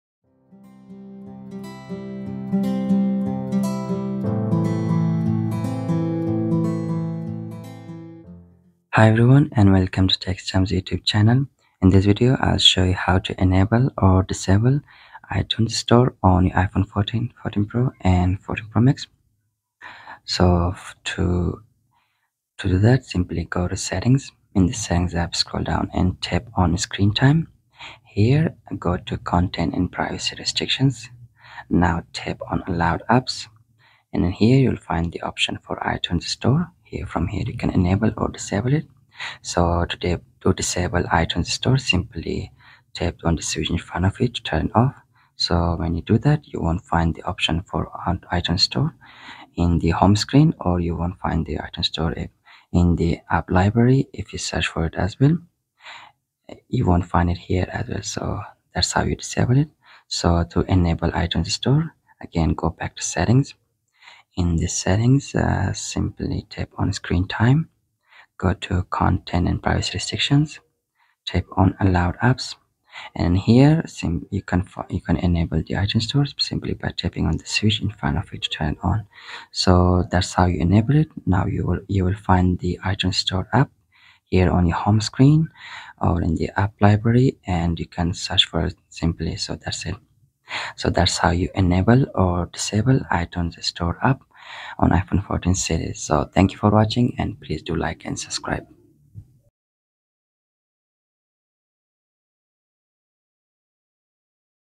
How To Enable/Disable iTunes Store on iPhone 14 Pro/Max
In this video, I will show you how to enable or disable the iTunes Store app on your iPhone 14, 14 Pro, and 14 Pro Max.